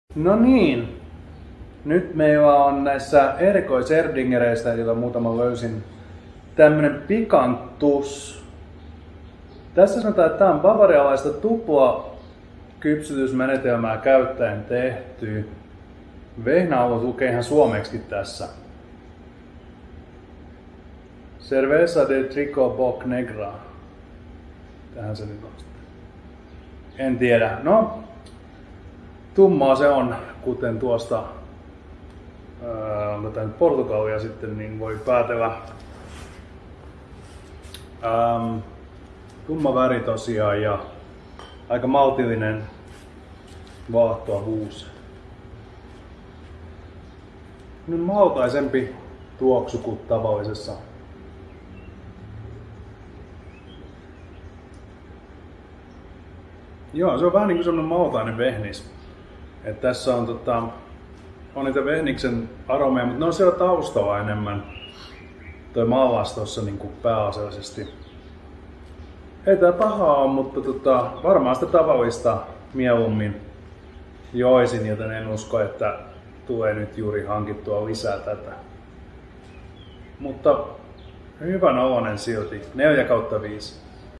Kepulla: Erdinger: Pikantus
Saksa
Vehnis

500ml
7.3%

Väri: Tumma, maltillinen vaahtoavuus
Aromi: Maltainen vehnis
Maku: mallas pääasiallisesti, vehnis taustalla
4/5
Hyvän oloinen mutta joisin tavallista mielummin